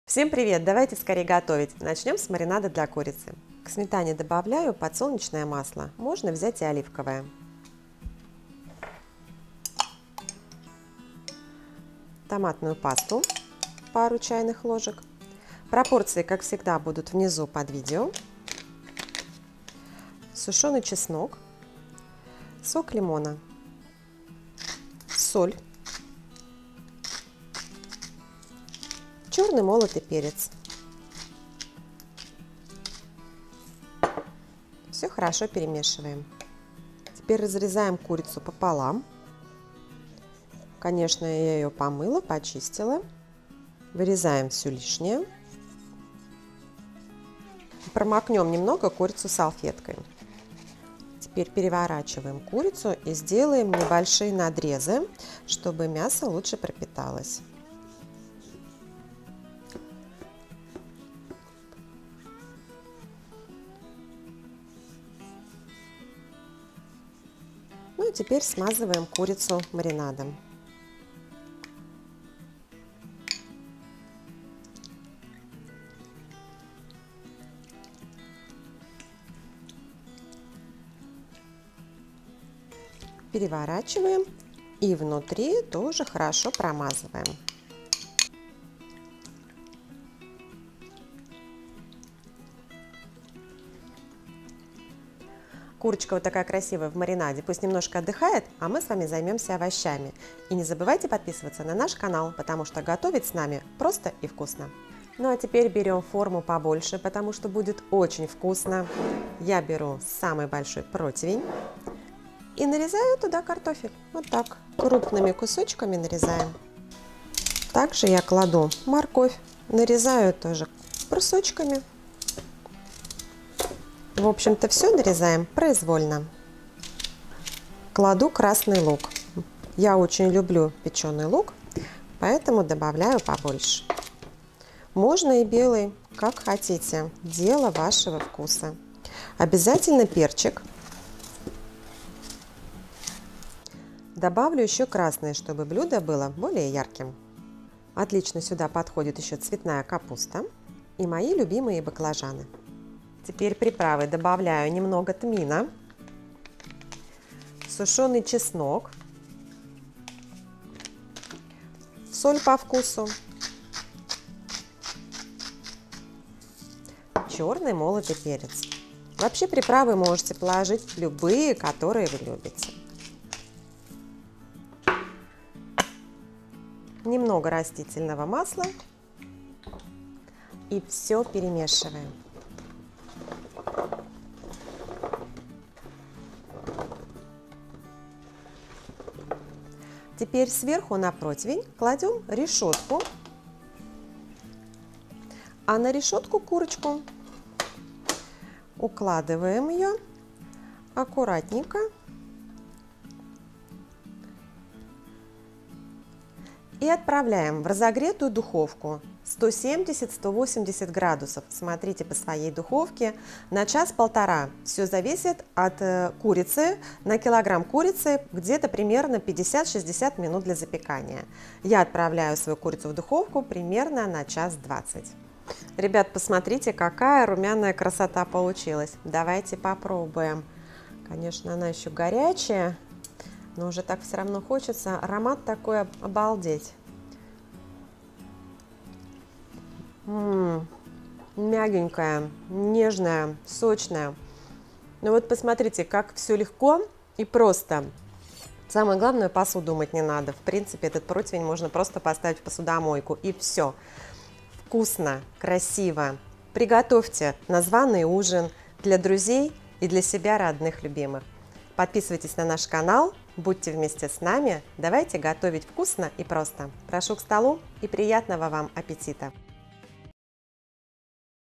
Это Мой Любимый Рецепт! Курица в Духовке с Овощами!
Это мой любимый рецепт! Цыпленок в духовке! Курица с овощами очень вкусное и простое блюдо!

1.5кг курица
5-6шт. картофель
1-2шт. морковь
1 баклажан
1 цветная капуста
2шт. лук репчатый
соль, перец по вкусу
1ч.л тмин
1ч.л сушеный чеснок

Маринад для курицы:
2ст.л сметаны или йогурт без добавок
2ст.л растительное масло
2ч.л томатной пасты
2ч.л сок лимона
1ч.л сушеный чеснок
1ч.л соль